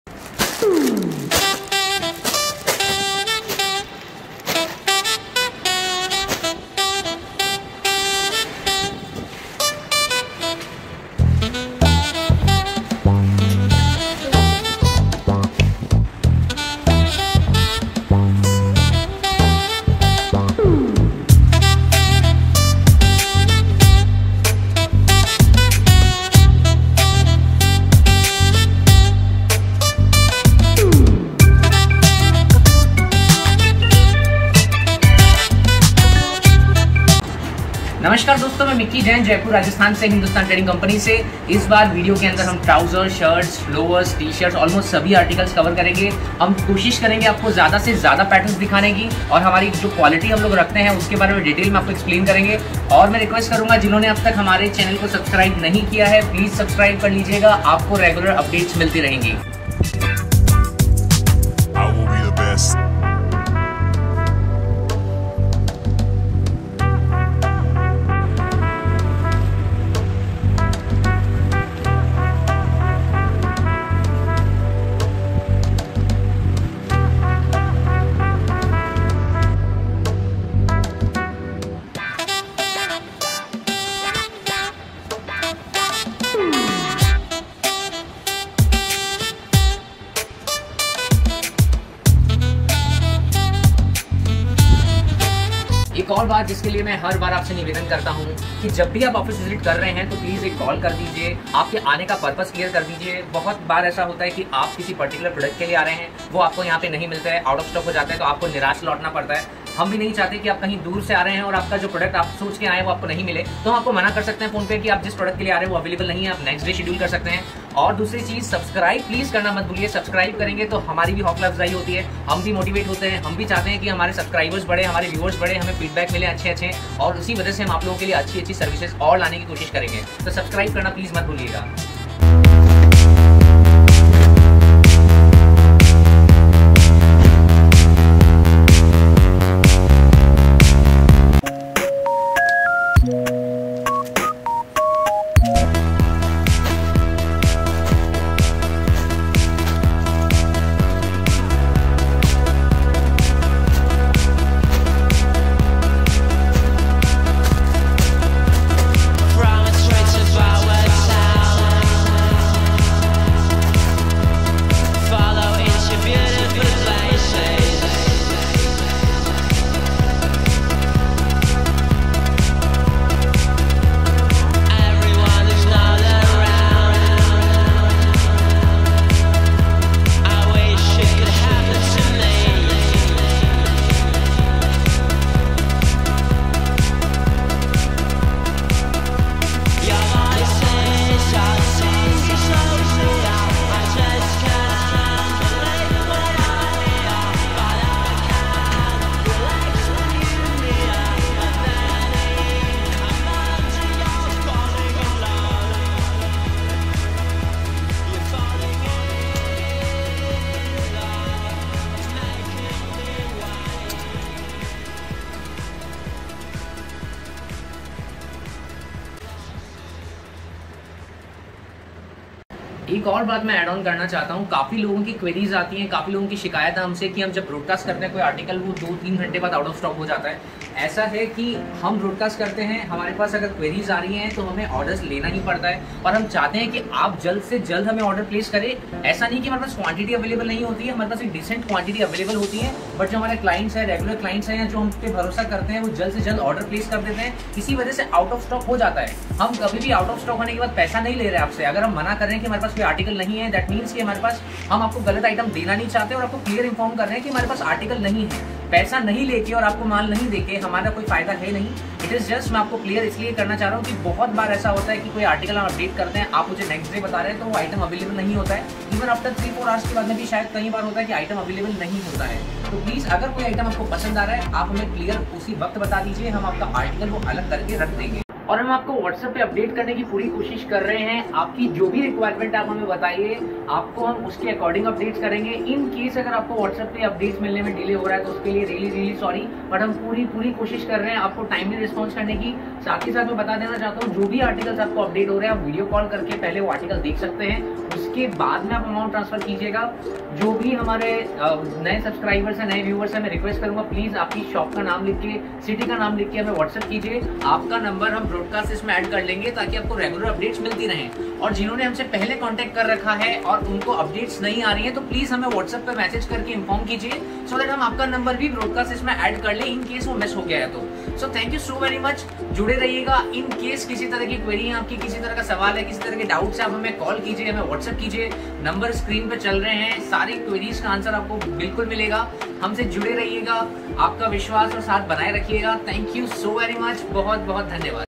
Complete Men's wear | Jaipur | Shirt, Jean, Trouser, Lower | Manufacturer & Wholesaler | 9024349754
Video Heighlights :-
:- Complete Men's Wear
:- Shirts, Jeans, Trousers, Lowers, Shorts

(Please do not call or message for single pieces)

Interested People can Visit or make a video call to check all our articles live.

Regards,
Hindustan Trading Company,
Jaipur.